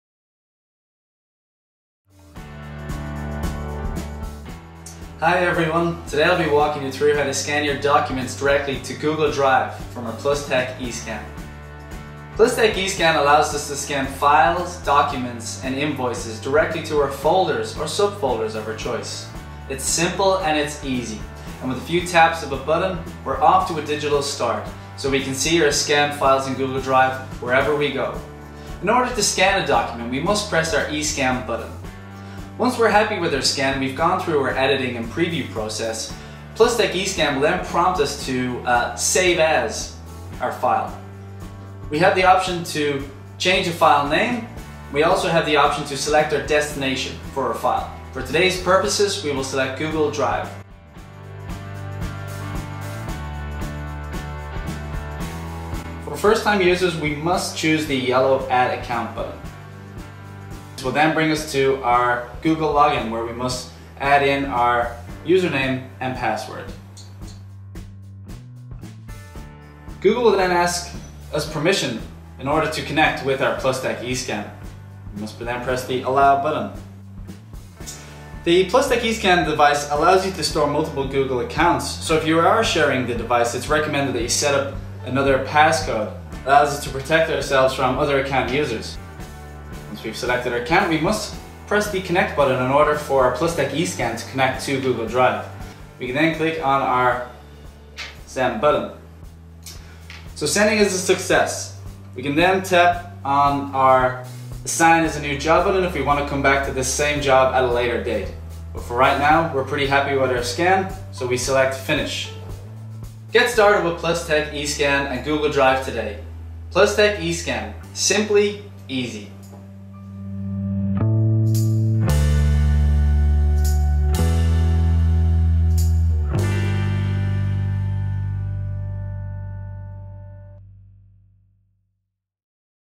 eScan Essential Tutorial-How to Scan directly to Google Drive from Plustek eScan?
Plustek eScan allows you to scan files, documents, and invoices and save them directly to Google Drive. The process is simple and easy, with a few touches of a button, you are off to a digital start. See your scan files in Google Drive, wherever you go.

--Plustek eScan to Google Drive EN 531VE-02201--